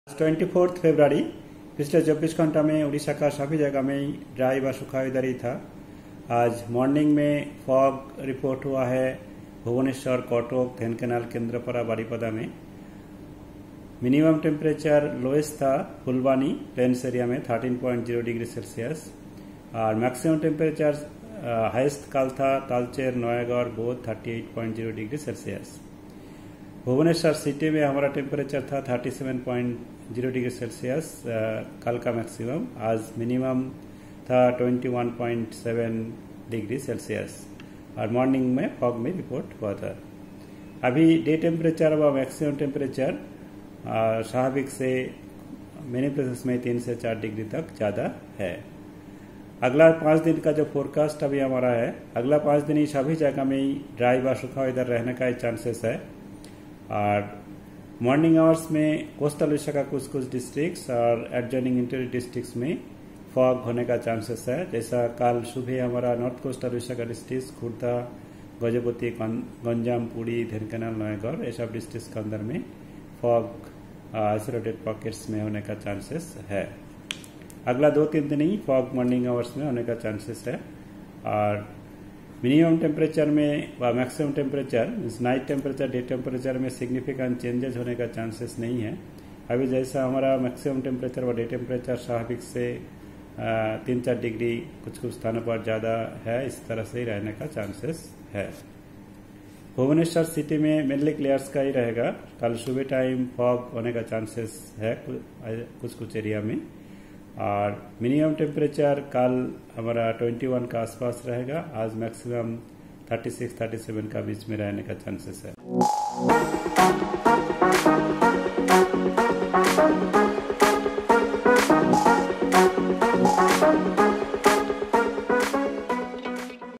IMD Predicts Chances of Fog in Coastal Odisha | Bhubaneswar Panipaga Suchana | Odisha Weather Update
As per IMD there are a few chances of fog in coastal parts of Odisha.